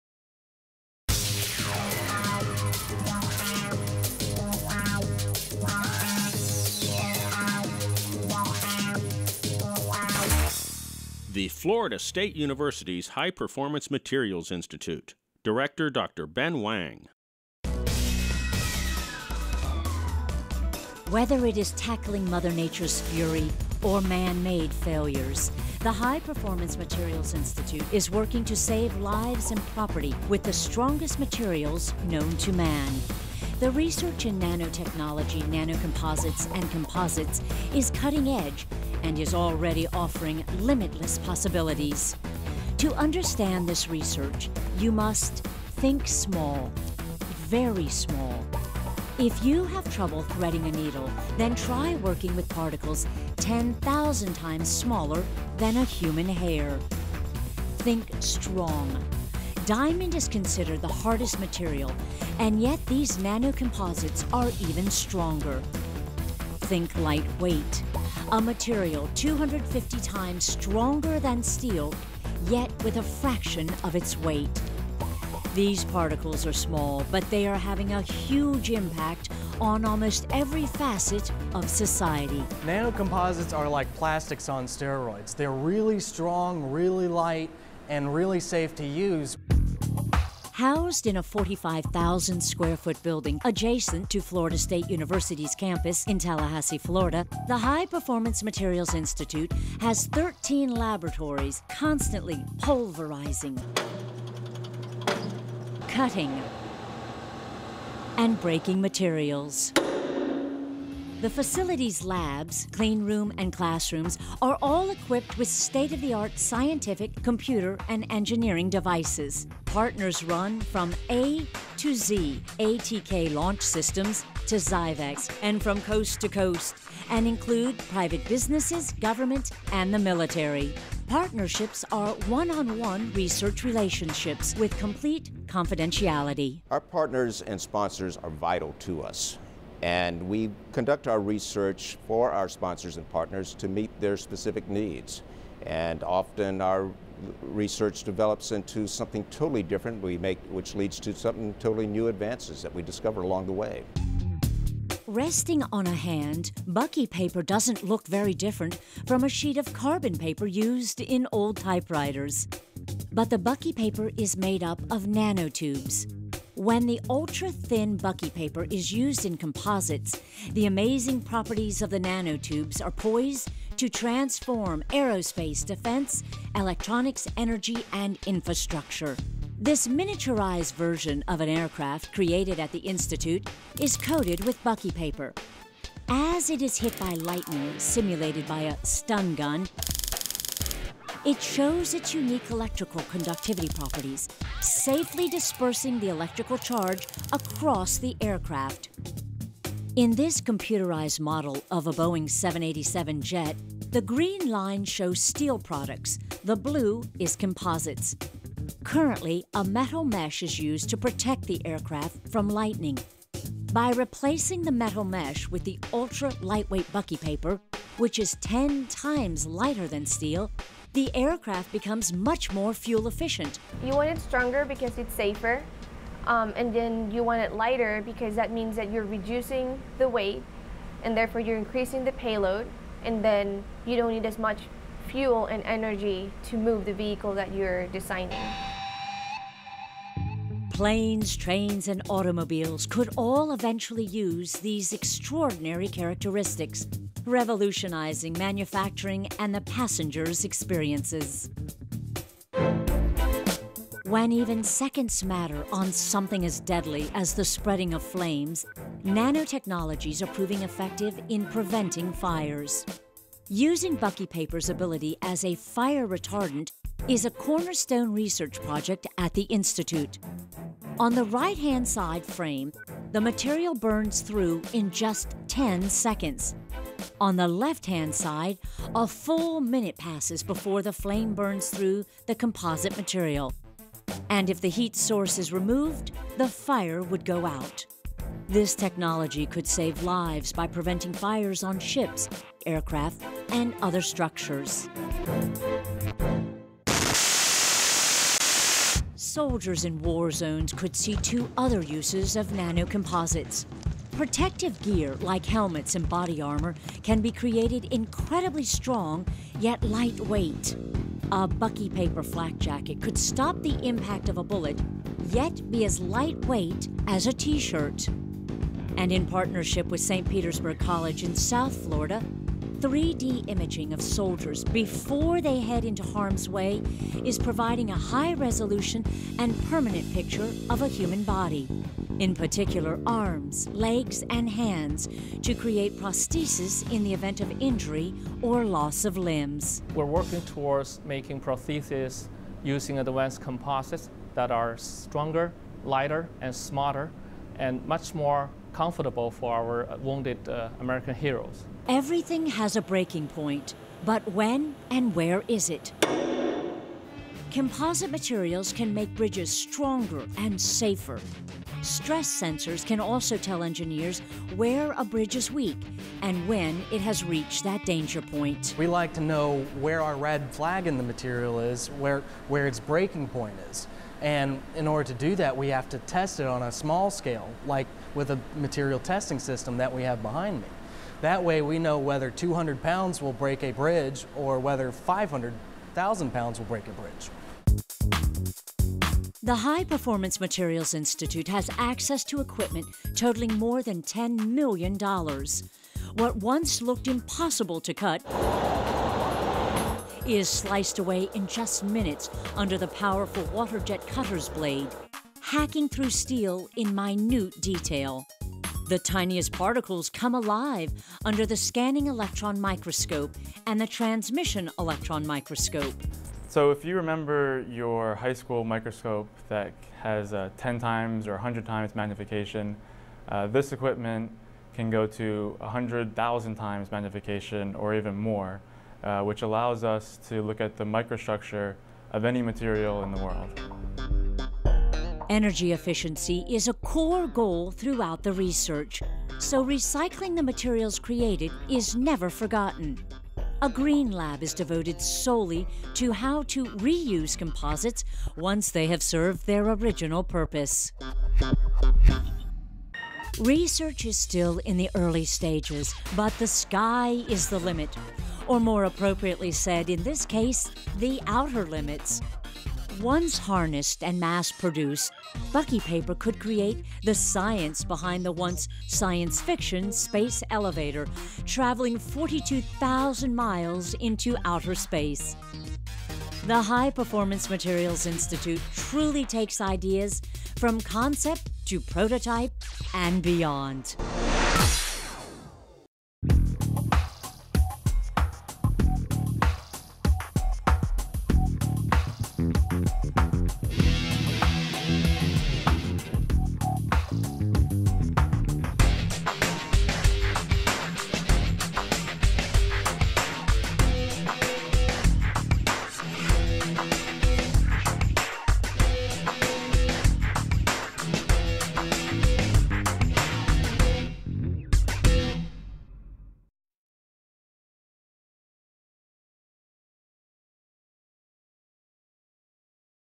High-Performance Materials Institute at Florida State University
The High-Performance Materials Institute is a Florida State University center that focuses on conducting a broad range of research and integration to improve the performance and affordability of advanced composite materials, components, subsystems and structures; expanding the pool of well-trained engineers and scientists for a technology sector of national and global significance; and working with tech transfer and economic development professionals in transferring and commercializing composite technology and increasing economic impact. The FSU HPMI research team has developed unique experience and expertise in the areas of nanoparticle dispersion, nanotube thin film or buckypaper materials and high performance nanocomposites. The HPMI team has also developed a state-of-the-art nanomaterial research infrastructure and labs in the new $21M, 45,000 square foot FSU Materials Research Building containing equipment valued at over $10M.